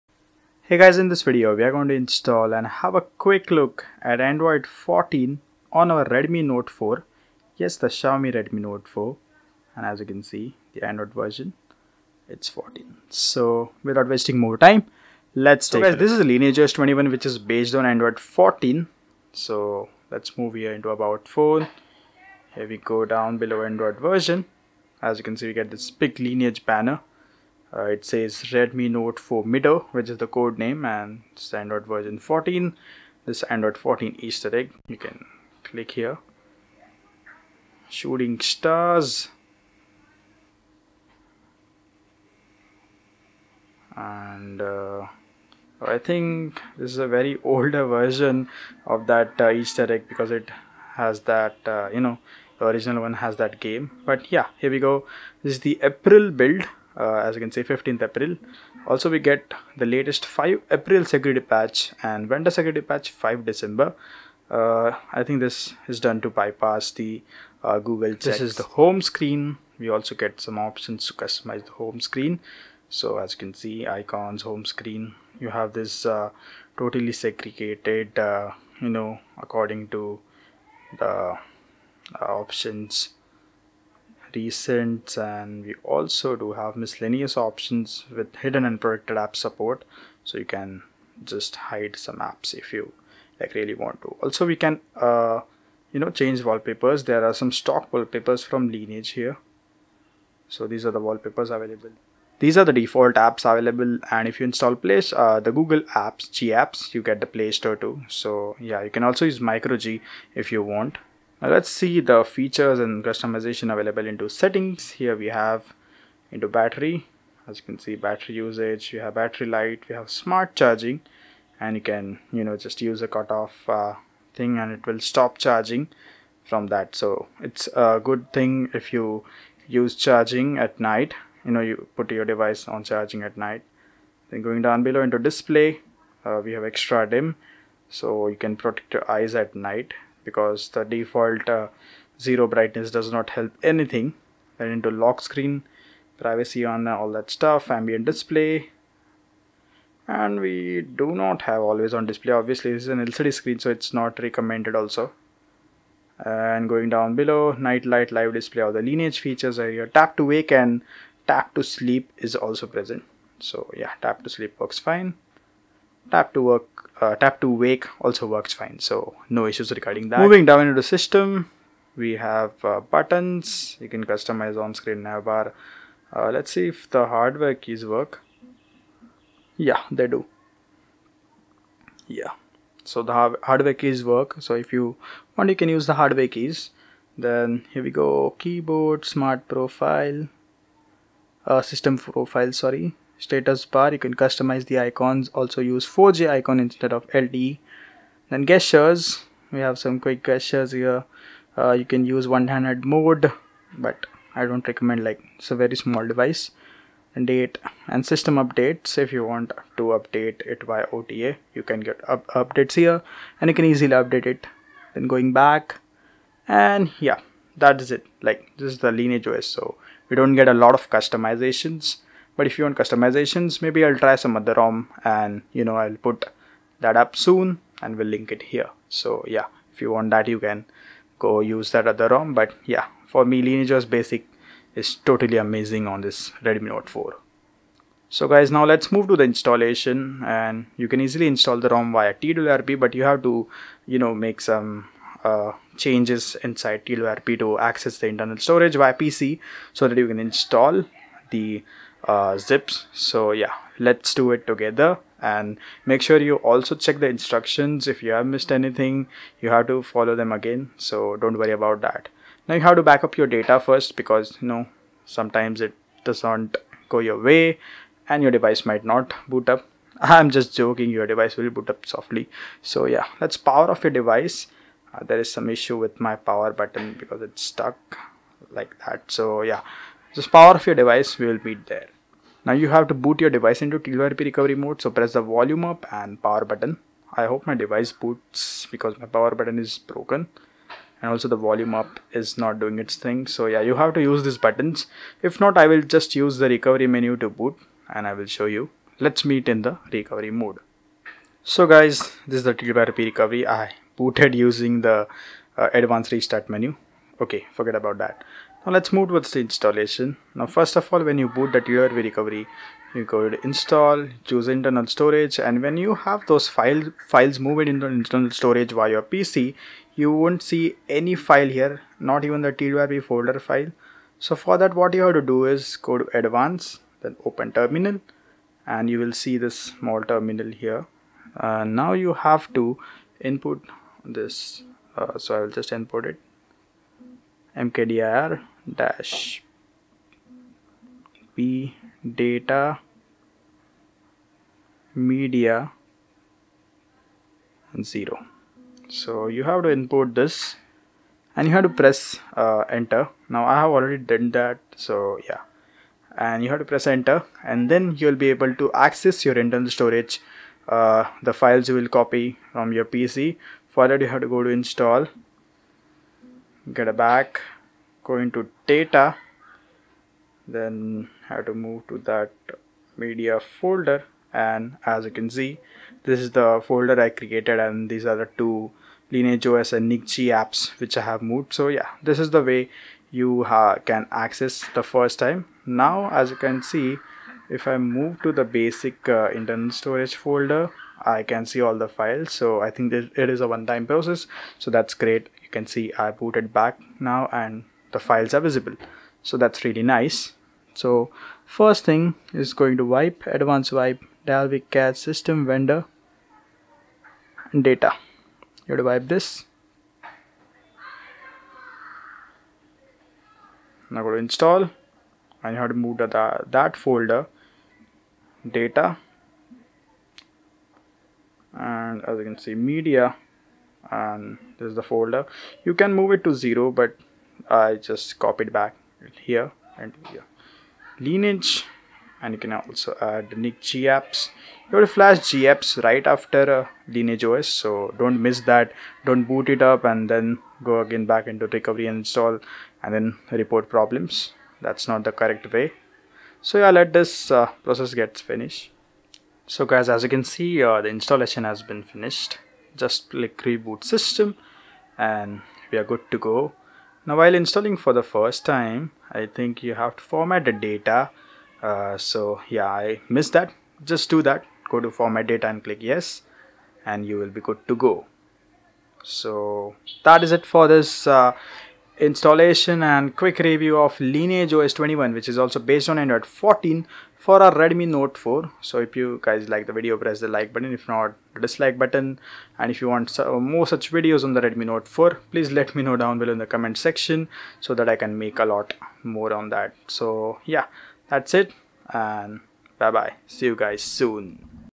Install Android 14 On Redmi Note 4!
LineageOS 21 brings Android 14 support to our Redmi Note 4. This custom ROM is stable and perfect for daily usage. It also comes with the latest April security patch with OTA updater support.

Credits - zeelog

Installation Instruction - Because of difference in regular installation process, follow installation instructions from video for step by step assistance.

Samsung Galaxy S7 - Sale Discontinued
Samsung Galaxy S8 - Sale Discontinued
OnePlus 6 - Sale Discontinued
Samsung Galaxy Watch4